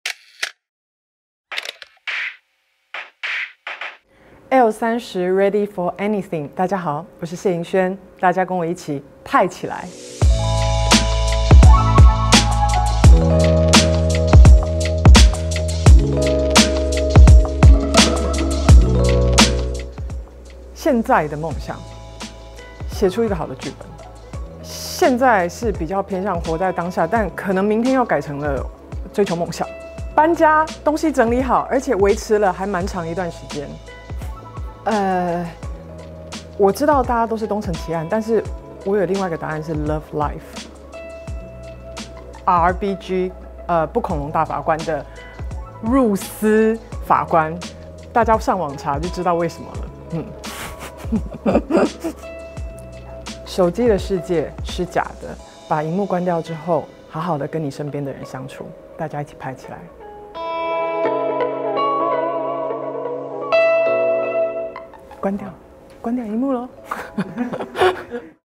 派起來之謝盈萱快問快答！她覺得最派的人，竟然是這位法官？！｜ELLE Taiwan
謝盈萱再扮《俗女》：「真實人生不是IG突然冒出一個金句，就能找到解答，那都是假的。」